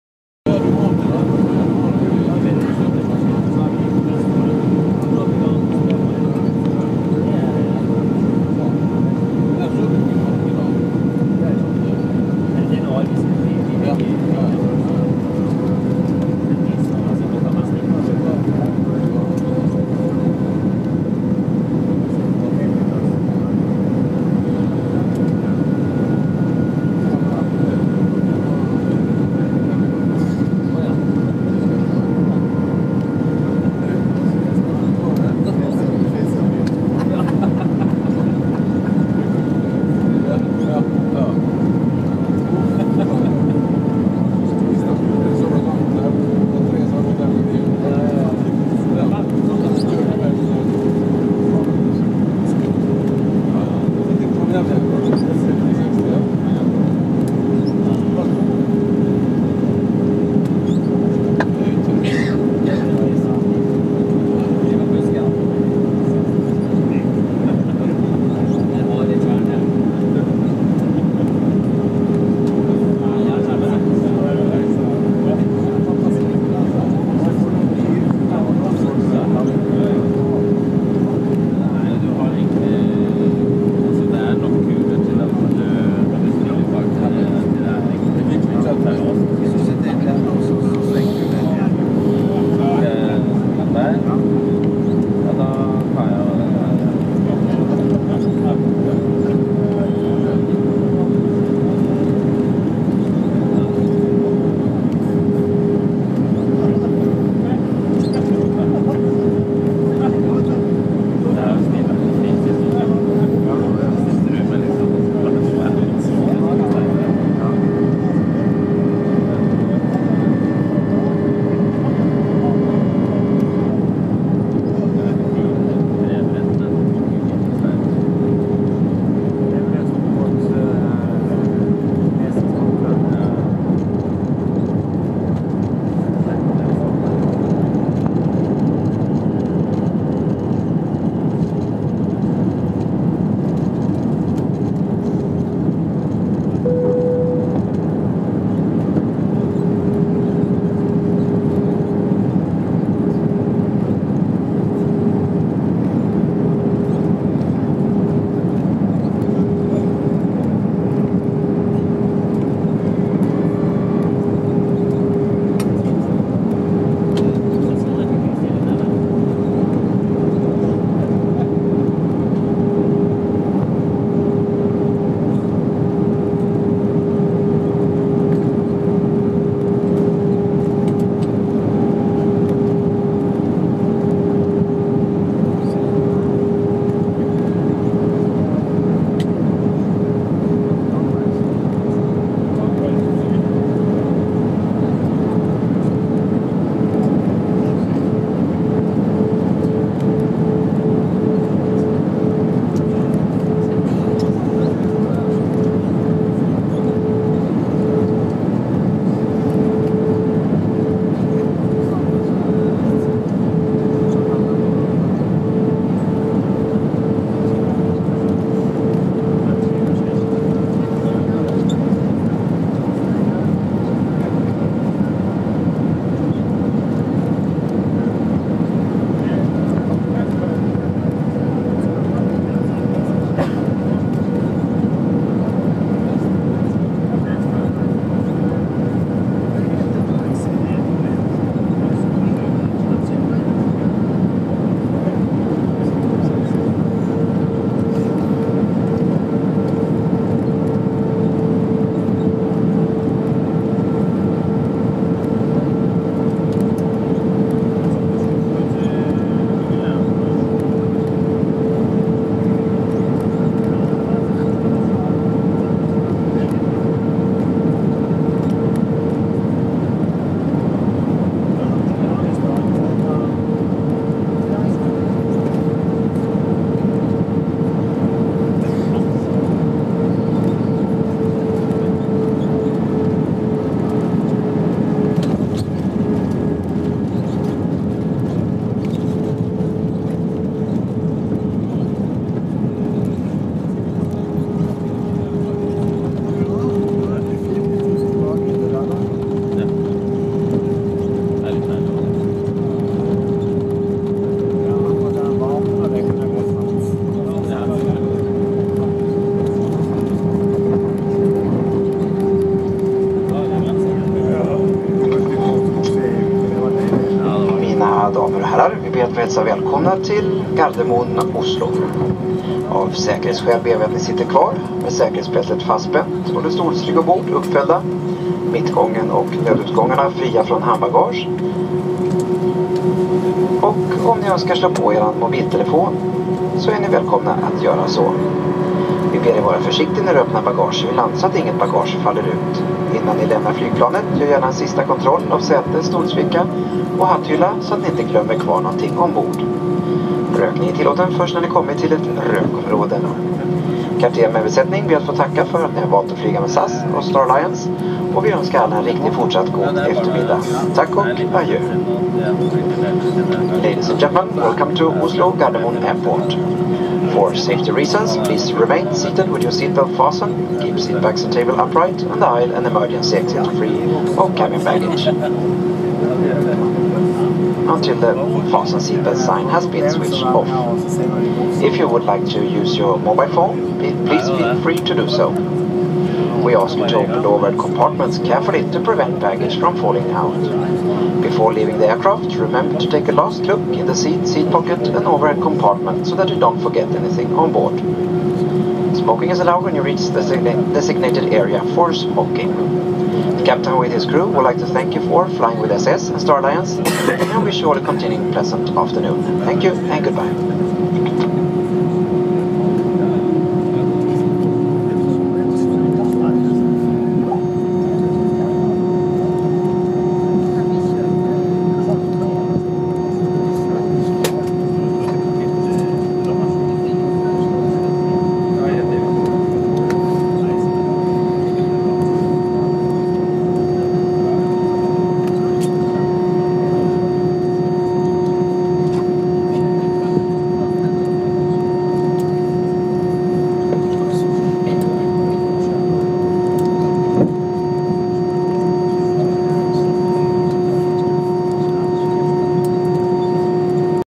Oslo, Final descent and landing in Oslo, SK 4756 FRA OSL, Runway 01L Airbus A 320, SE RJ
Oslo, Final descent and landing in Oslo,
Flight SK 4756, Frankfurt (FRA) - Oslo (OSL), Runway 01L,
Airbus A 320, SE-RJ
2015|08|26